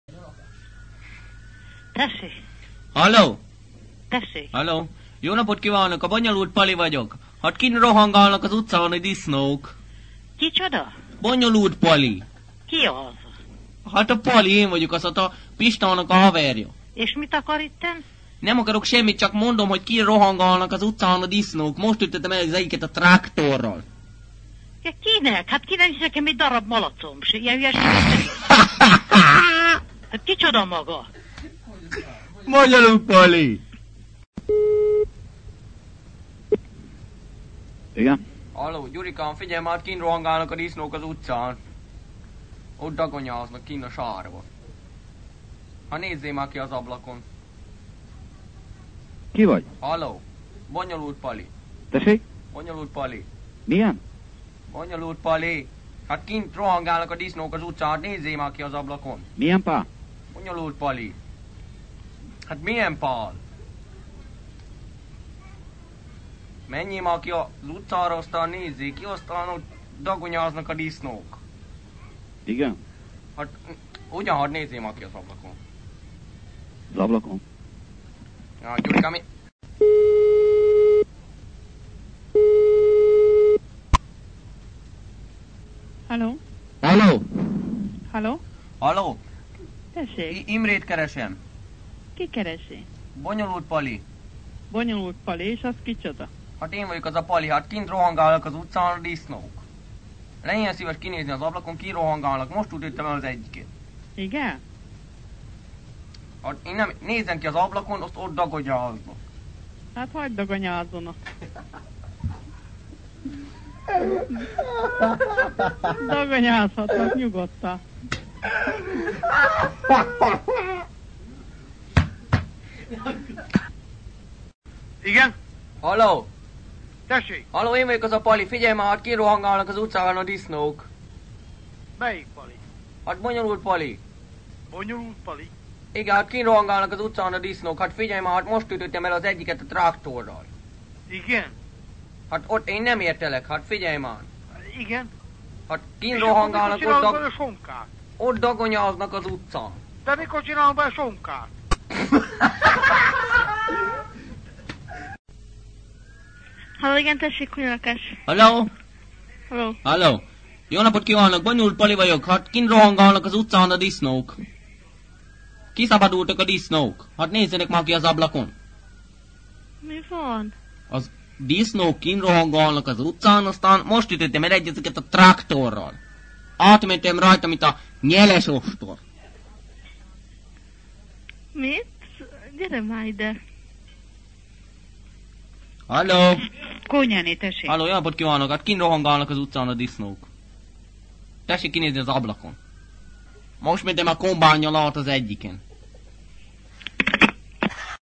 Telefonbetyár - Bonyolult Pali
Bankszámla:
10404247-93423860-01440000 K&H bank

Békési telefonbetyárok a 90-es években valamikor. Kazettán kaptam meg tőlük eredetileg,  digitalizálva is lett és pár el is terjedt a neten, de most a kazit is megtaláltam, amin eredetileg volt digitalizáltam újra.  Van amelyik elég gyenge lett, de ha már van közkinccsé teszem mindet, anno is többet felraktam a netre.